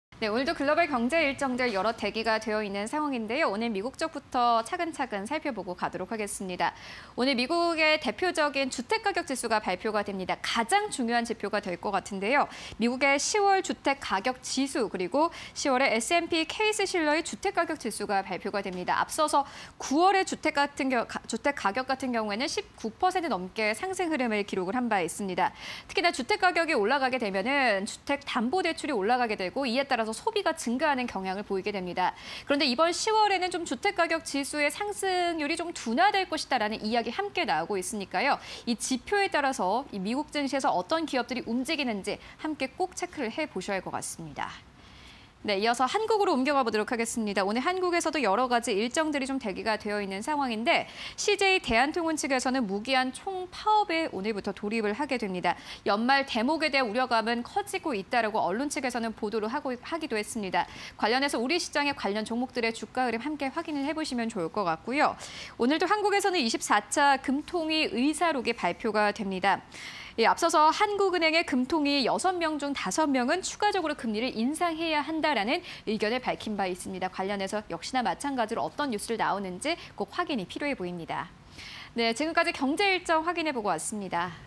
美 주택가격, 9월 19.1%↑… 관련 지표 주목_오늘장 글로벌 일정 (20211228)
미국 : 12월 리치몬드 연방 제조업지수
미국 : 10월 주택가격지수
미국 : 10월 S&P/케이스쉴러 주택가격지수
영국 : 증시 휴장

러시아 : 러시아-벨라루스 정상회담
한국 : 1월 소비자동향조사
한국 : CJ대한통운, 무기한 총파업 돌입
한국 : 21년 24차 금통위 의사록